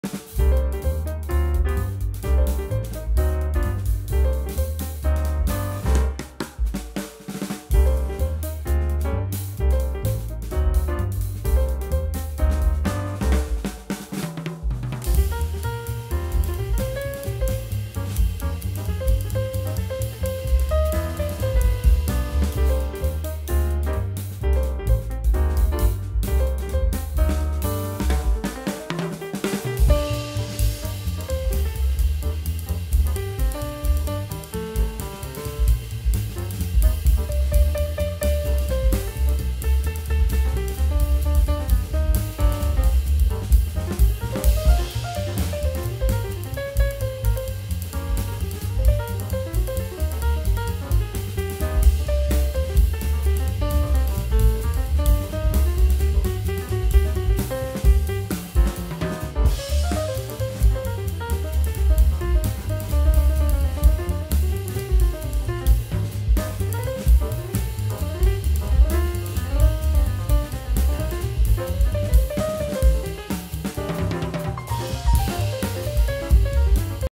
【From TokyoThree popular hot springs destinations for a day trip
【From Tokyo】Three popular hot springs destinations for a day trip

I hope this helps you all out!